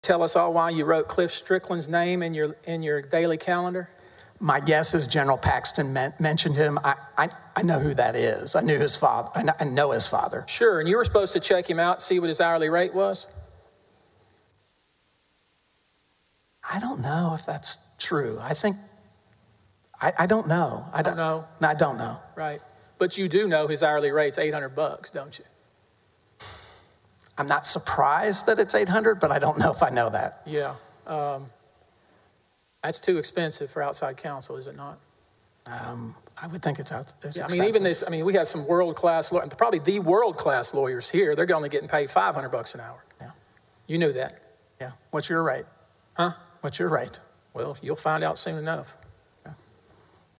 Paxton impeachment trial: Tony Buzbee trades jab with witness over hourly rate
Tony Buzbee and the witness traded a jab over hourly rate during questioning in the Ken Paxton impeachment trial.

You can follow live updates from the trial here on WFAA